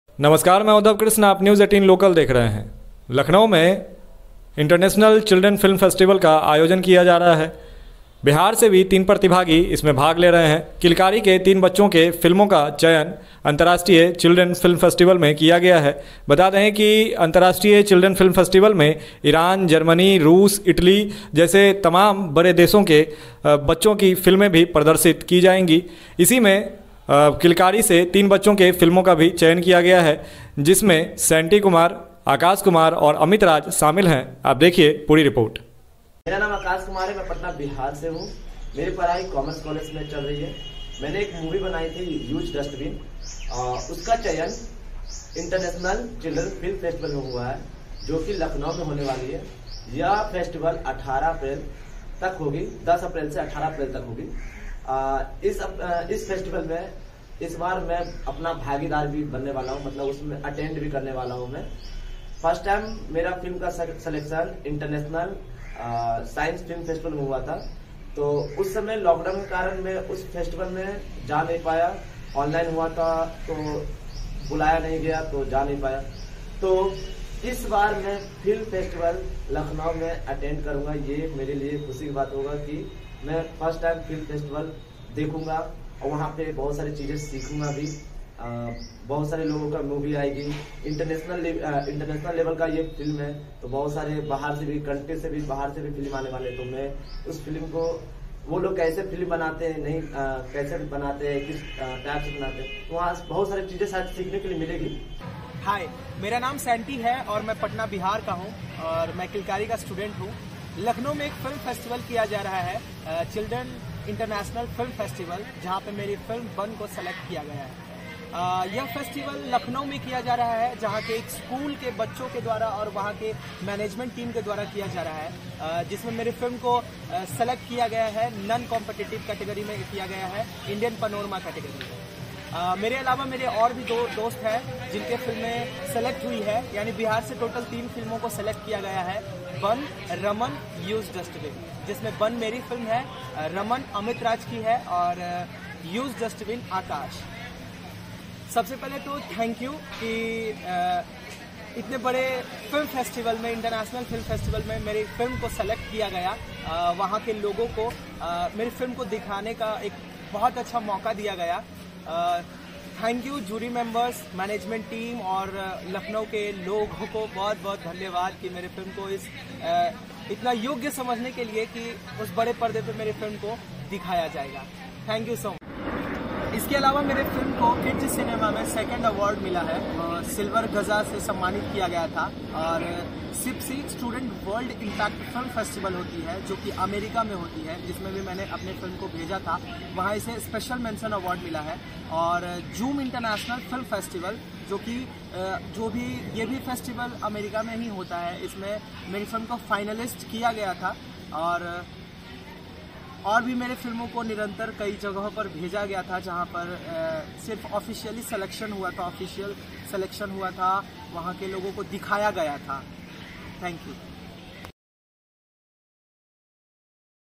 Patna news: चिल्ड्रन फिल्म फेस्टिवल में बिहार के तीन बच्चों की फिल्मों का होगा प्रदर्शन, कई बॉलीवुड
उत्तर प्रदेश की राजधानी लखनऊ में इंटरनेशनल फिल्म फेस्टिवल का आयोजन किया जाना है. 18 अप्रैल तक इसका आयोजन किया जाएगा. इसमें अमेरिका, ईरान, इंडोनेशिया, इटली और जर्मनी समेत भारत से कई बच्चों के फिल्म का प्रदर्शन होगा. बिहार से इसमें सैंटी कुमार, आकाश कुमार और अमित राज की फिल्म का चयन किया गया है.